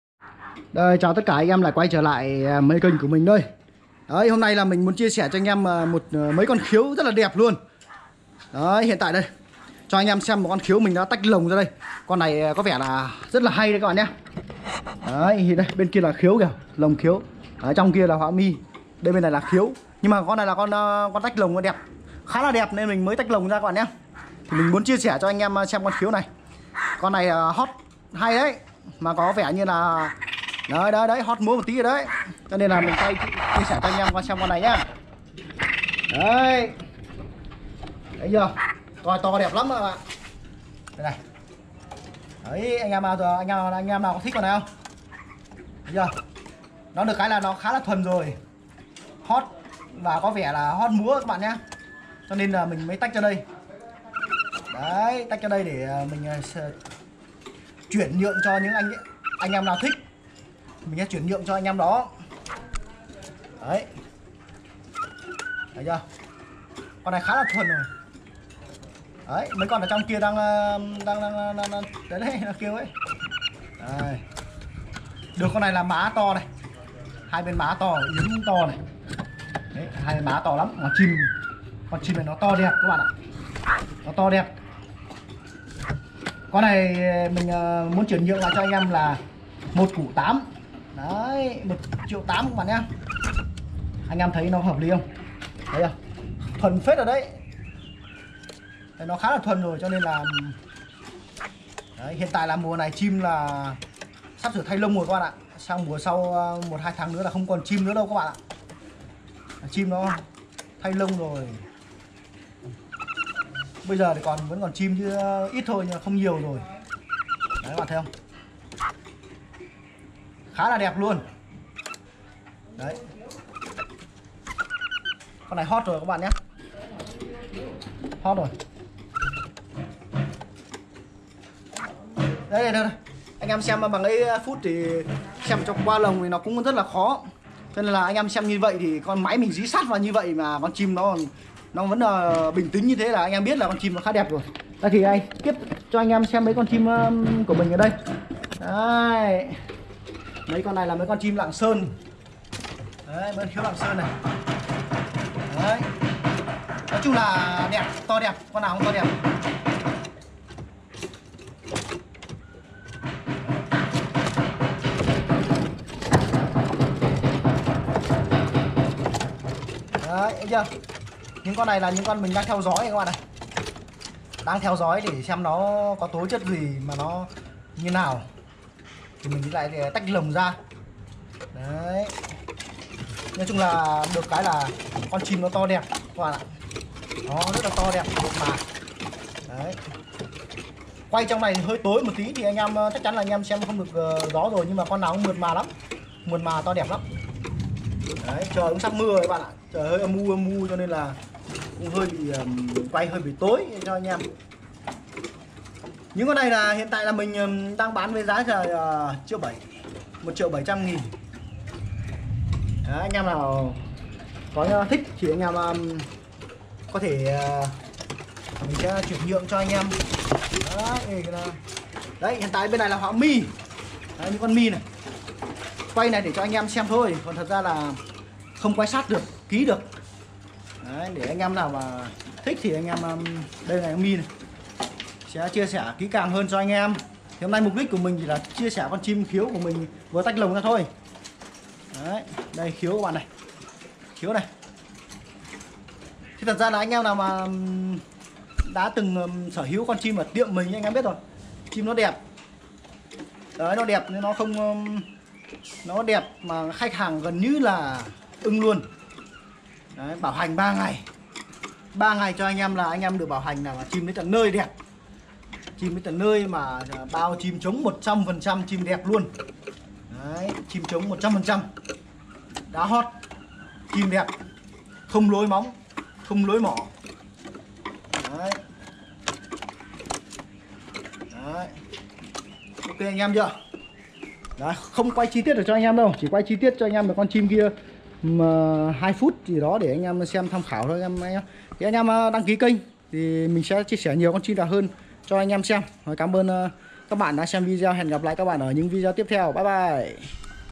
CHIM KHƯỚU ĐẸP TÁCH LỒNG HÓT MÚA MỜI CÁC BÁC XEM ĐƯỢC KHÔNG Ạ
MỜI CÁC BẠN XEM CHON CHIM KHƯỚU ĐẸP HÓT MÚA CÙNG NGUYỄN LỢI TV